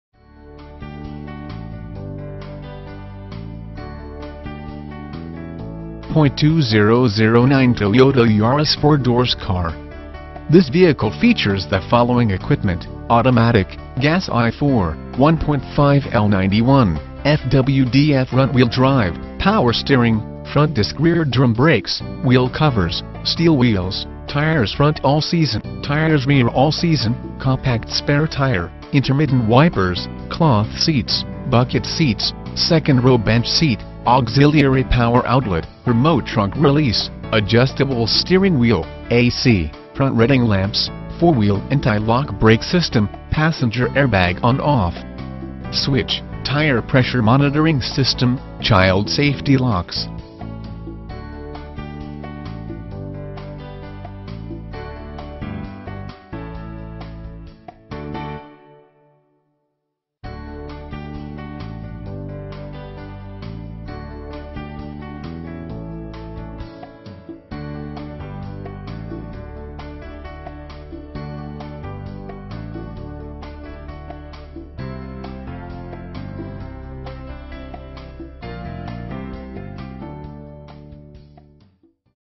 2009 Toyota Yaris - Classic Dealer Group
For Sale: 2009 Toyota Yaris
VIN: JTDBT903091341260
Engine: Gas I4 1.5L/91
Drivetrain: FWD
Transmission: Automatic
Mileage: 43,016
Color: Silver Streak Mica (exterior) Dark charcoal (interior)

Features:
2009 Toyota Yaris four doors Car.